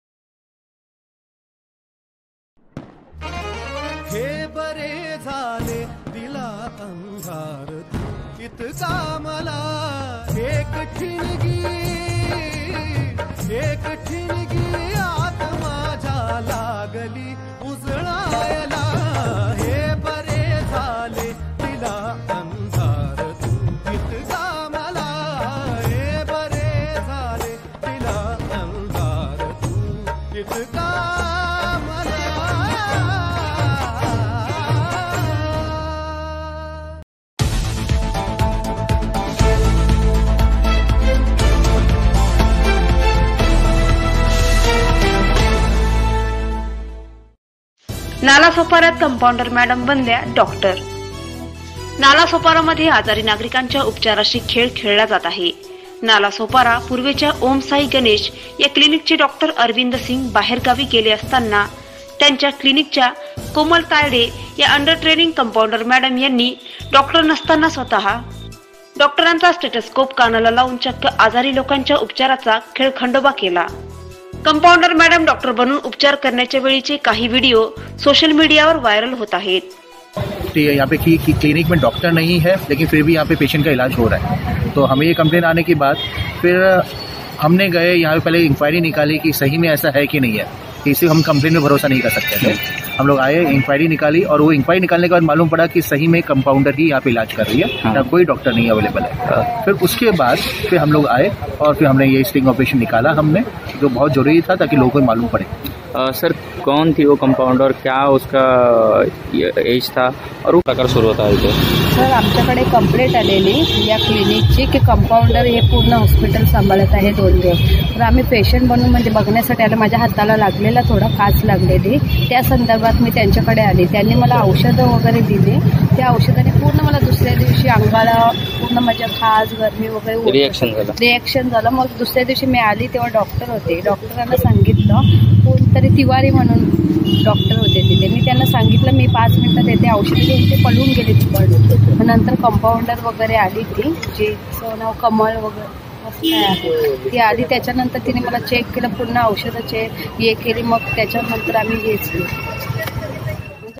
कंपाउंडर मॅडम बनल्या डॉक्टर...आजारी नागरिकांच्या जीवाशी खेळ...
नालसोपरा मध्ये आजारी नागरिकांच्या उपचाराशी खेळ खेळला जात आहे.
नालासोपारा पूर्वेच्या ओम साई गणेश या क्लिनिक चे डॉक्टर अरविंद सिंग बाहेर गावी गेले असताना त्यांच्या किल्निक च्या कोमल तायडे ह्या अंडर ट्रेनींग कंपाउंडर मॅडम यांनी डॉक्टर नसताना स्वता डॉक्टरांचा सेट्सकोप कानाला लावून चक्क आजारी लोकांनच्या उपचाराचा खेळ खंडोबा करण्यात लागल्या आहेत . कंपाउंडर मॅडम डॉक्टर बनून उपचार कर्त्या वेळी चे काही व्हिडिओ सोशल मीडियावर व्हायरल होत आहेत .